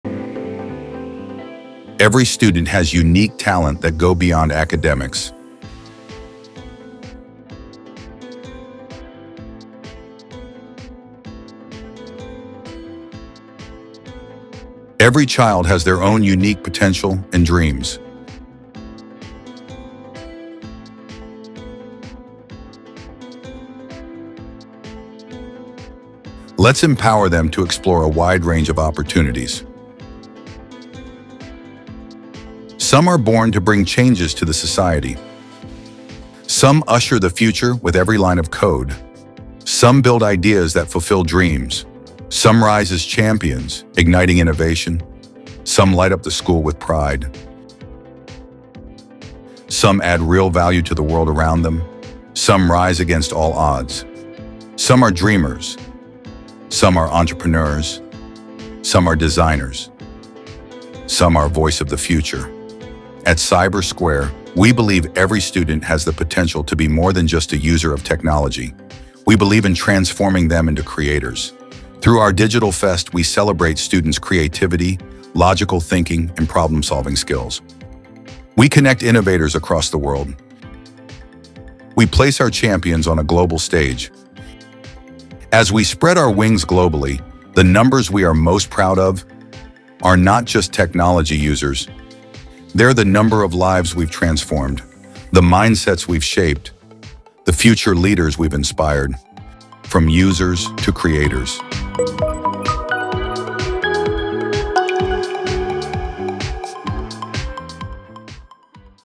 From tech users to tech creators — this is how Cyber Square transforms the future
At Cyber Square, we believe that every student has the potential to become a creator of technology, not just a user.
This video showcases the real impact of introducing coding, artificial intelligence, and robotics into school curricula. Watch how students confidently explore, create, and innovate inside our smart tech labs!

👩‍💻 Students are not just learning—they’re:
🔹 Building games and animations
🔹 Programming robots
🔹 Developing problem-solving and logical thinking
🔹 Preparing for a tech-driven future

✅ Powered by Cyber Square’s unique curriculum and hands-on training, schools across the world are transforming education for the digital age.